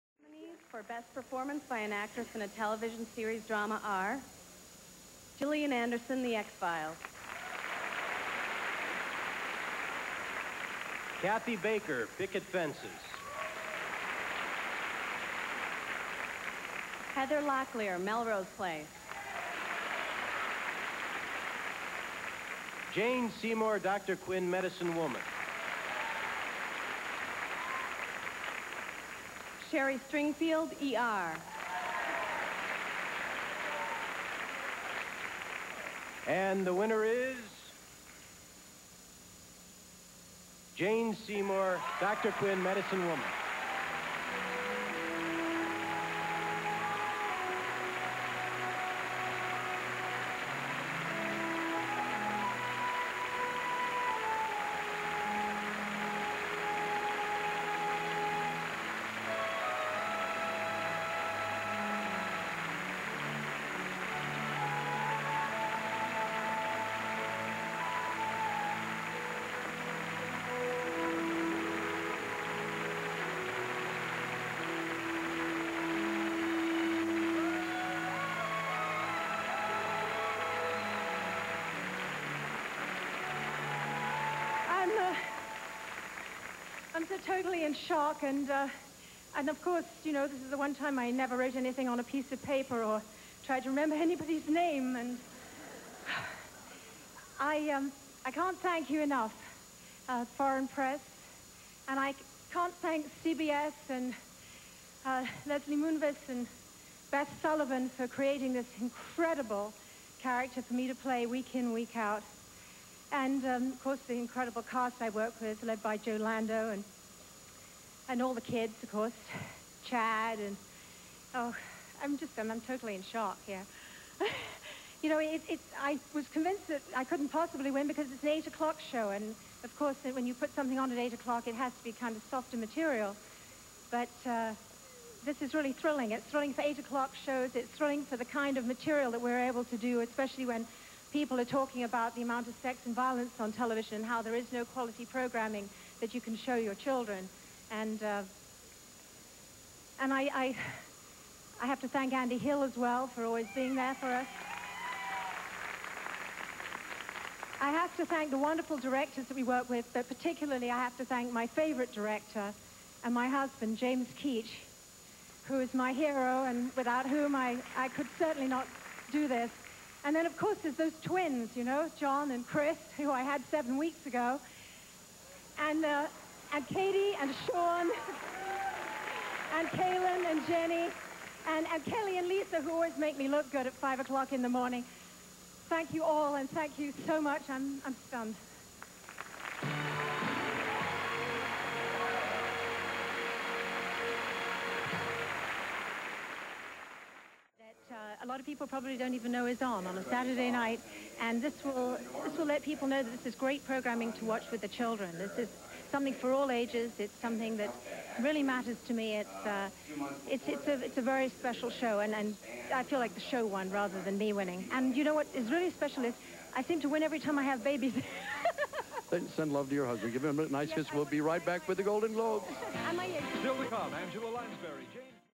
Jane Seymour wins Golden Globe for DR. QUINN [2.17]
Jane Seymour's Golden Globe win for Best Performance by an Actress in a TV-Series - Drama, for DR. QUINN, MEDICINE WOMAN, in 1996.  She was also nominated in 1994, 1995, and 1997.  Also included are her comments backstage with Dick Clark.  (Transcript below.)

TRANSCRIPT:

[Jane]  I'm, ah... I'm so totally in shock.  And, of course, this is the one time that I never wrote anything on a piece of paper, or tried to remember anybody's name.  I... I can't thank you enough, Foreign Press, and I can't thank CBS and Leslie Moonves, and Beth Sullivan for creating this incredible character for me to play, week in, week out.  And, of course, the incredible cast that I work with, led by Joe Lando, and all the kids, of course... Chad.  I'm totally in shock.  You know, I was convinced that I couldn't possibly win because it's an 8:00 show, and, of course, when you put something on at 8:00, it has to be kind of softer material, but this is really thrilling.  It's thrilling for 8:00 shows, it's thrilling for the kind of material that we're able to do, especially when people are talking about the amount of sex and violence on television, how there is no quality programming that you can show your children.  And I have to thank Andy Hill, as well, for always being there for us.  I have to thank the wonderful directors that we work with, but particularly I have to thank my favorite director, and my husband, James Keach, who is my hero, and without whom I could certainly not do this.  And then of course, there's those twins... John & Kris, who I had seven weeks ago, and Katie, and Sean, and Kalen, and Jenni, and Kelly & Lesa who always make me look good at 5:00 in the morning.  Thank you all and thank you so much.  I'm stunned.

[Jane] .that a lot of people probably don't know is on, on a Saturday night, and this will let people know that this is great programming to watch with their children. This is something for all ages, it's something that really matters to me. It's a very special show and I feel like the show won, rather than me winning. And you know what's really special is, I seem to win every time I have babies.

[Dick]  Send love to your husband, give him a nice...

[Jane]  Yes, my husband... and my agent.

[Dick]  We'll be right back with the Golden Globes!